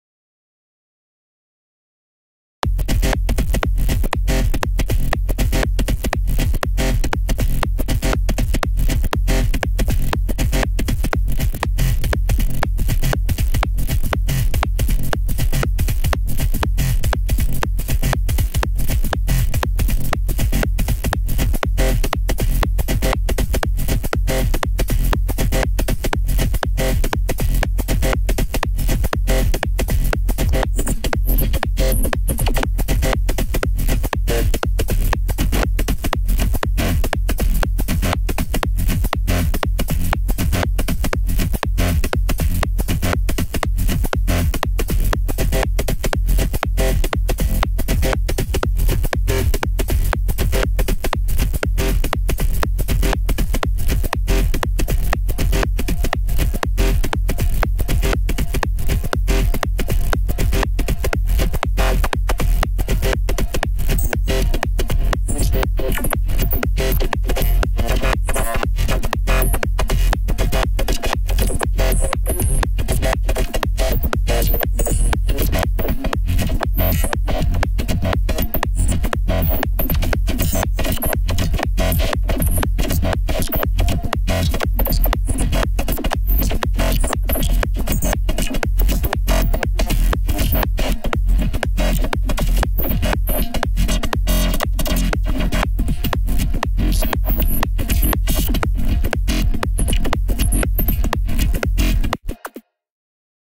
phaseplant is bonkers man
the following a single phaseplant patch i was messing with made entirely of raw synthesis and enough effects to blue screen my computer twice in the process of working on it